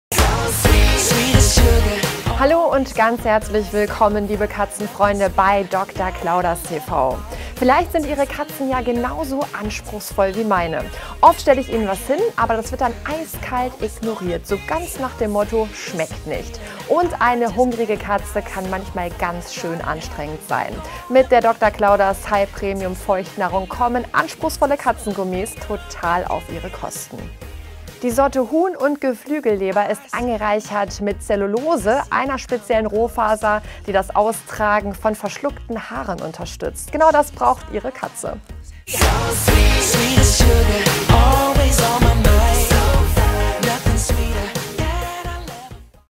Dr.Clauder´s Selected Pearls Huhn & Geflüegelleber für Katzen
Dr.Clauder's High Premium Selected Pearls Huhn & Geflügelleber ist die sinnliche Genuss-Komposition mit Antihairballeffekt durch speziellen Rohfaserzusatz. Ohne Farb- und Konservierungsstoffe. Alleinfuttermittel für ausgewachsene Katzen. Hergestellt in Deutschland und ohne medizinische Tierversuche entwickelt. Mehr Informationen finden Sie auf unserer Homepage